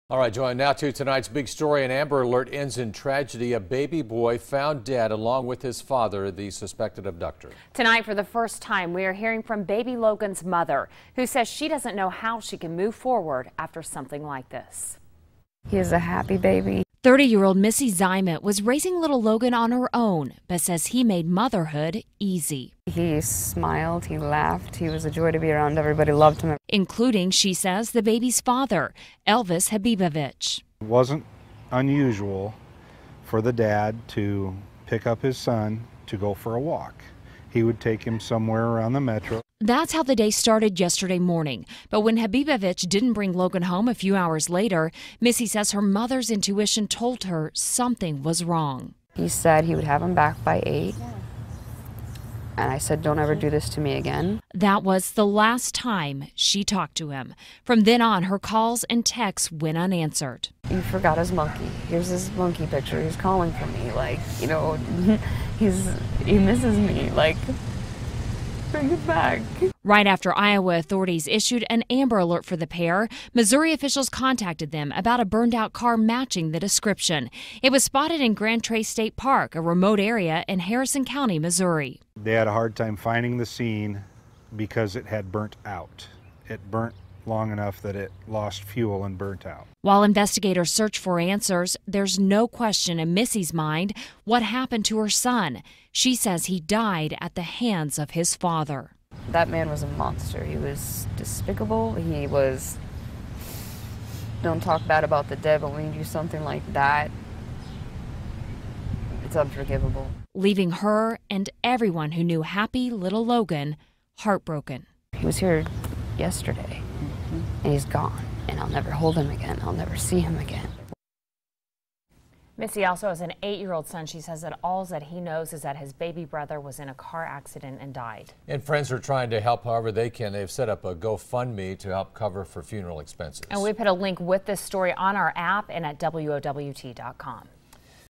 Amber Alert Child's Mother Heartbroken
Mother Speaks Out After Amber Alert Ends in Tragedy.